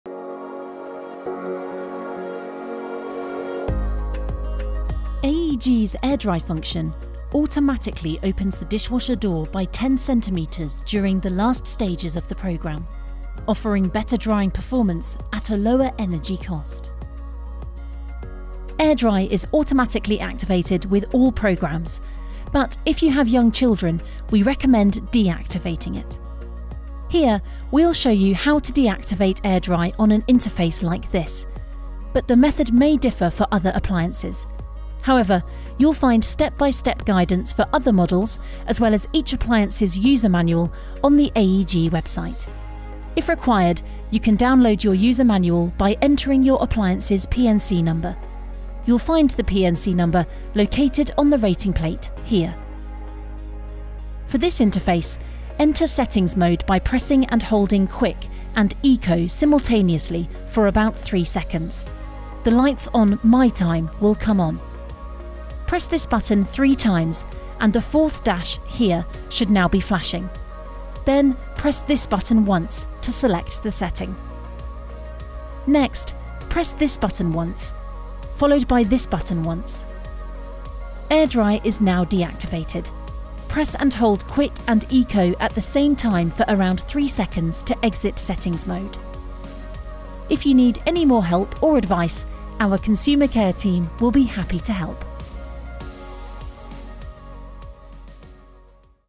How to activate Airdry automatic door opening on my AEG dishwasher
Learn how to activate the Airdry function on your AEG dishwasher, allowing you to dry your dishes in a more energy-smart and natural way.

For more tips and info on AEG dishwashers, check out our FAQs